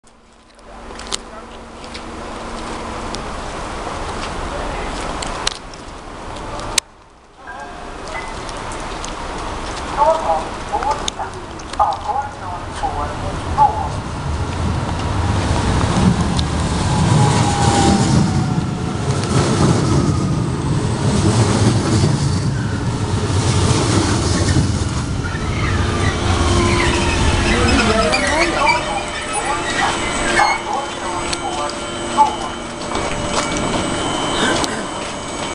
SL X60 Pendeltåg ankommer Sundbyberg Station (1)
Inspelat den 28 februari 2011.